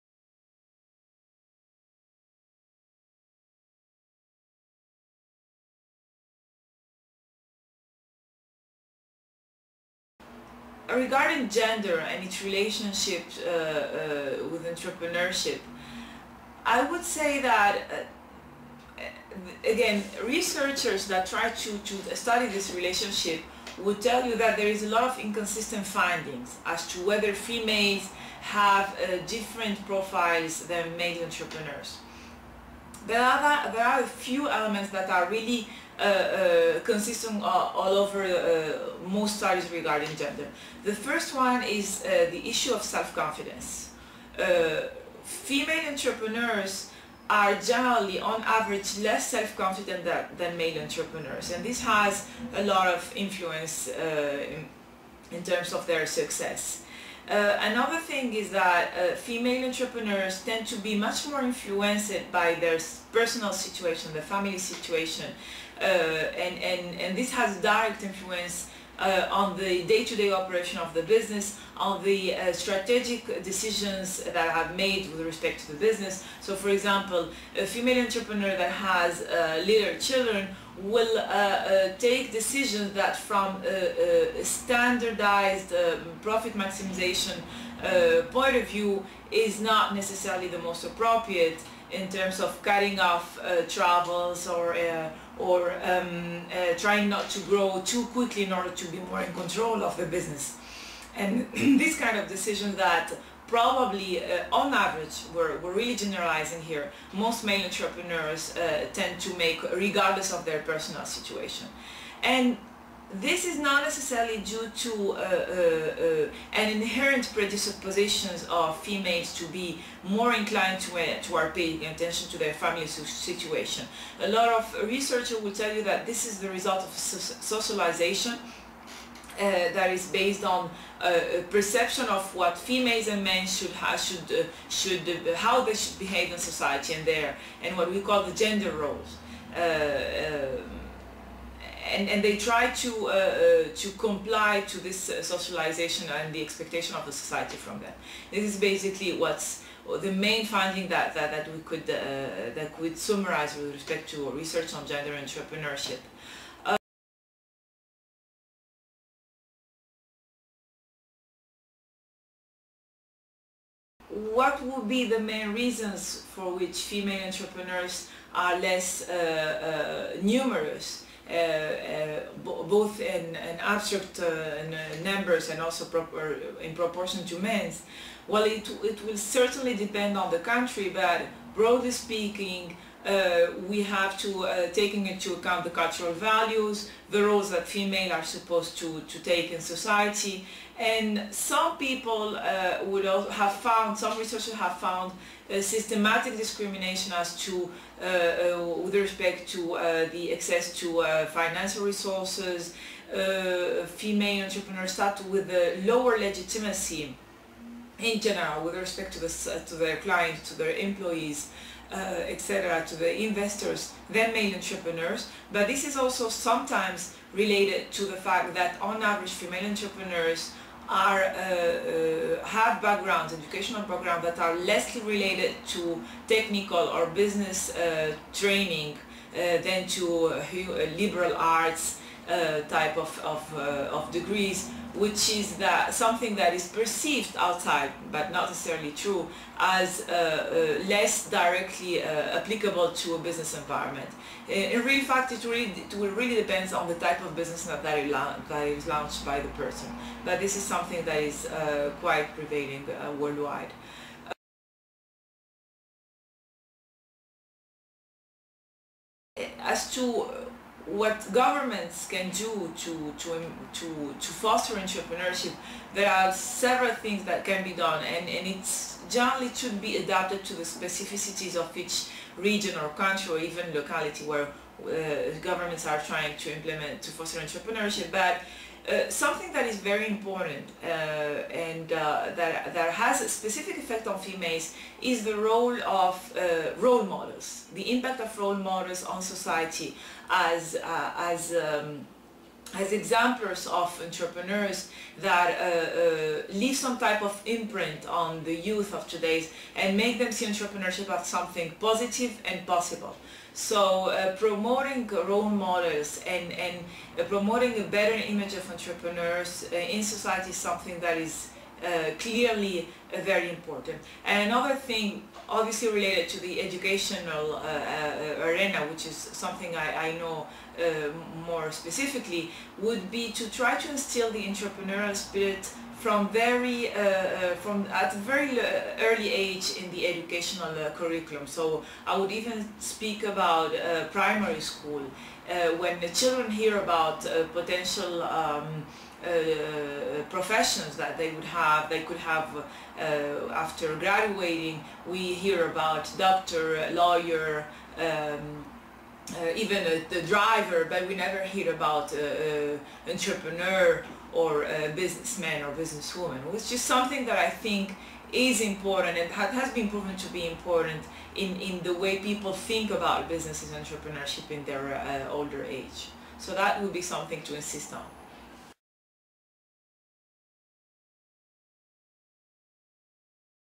Interview with Dr. Rachida Justo
IE School of Arts and Humanities interviews Dr.Rachida Justo about Women Entrepreneurship